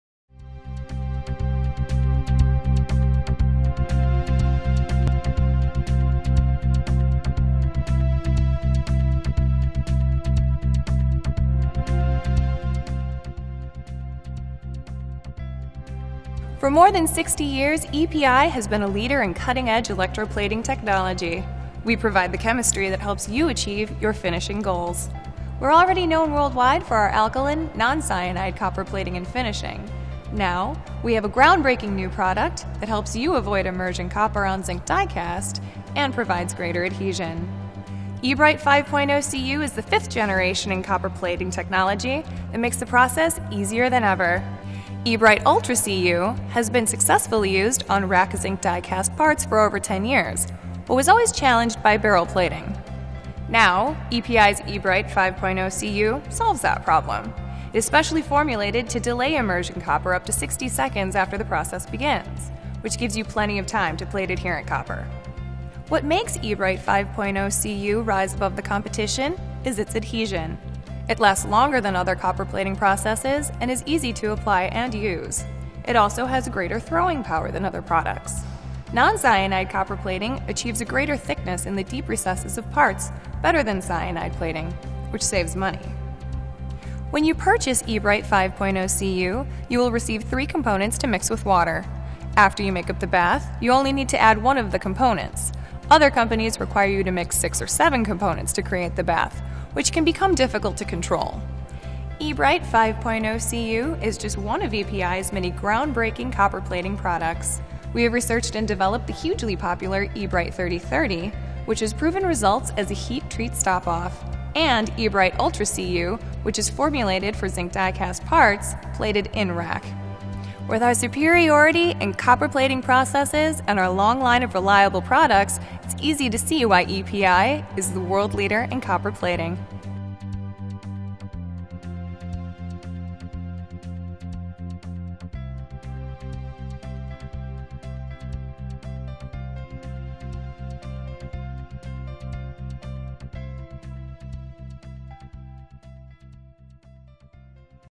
Alkaline non cyanide copper for barrel zinc diecast - E-Brite 5.0 Cu from EPi
Alkaline non cyanide copper that successfully plates on zamac/zinc diecast in barrel and rack without immersion copper problems like previous/other alkaline non cyanide copper processes. E-Brite 5.0 Cu is our fifth generation alkaline non cyanide copper.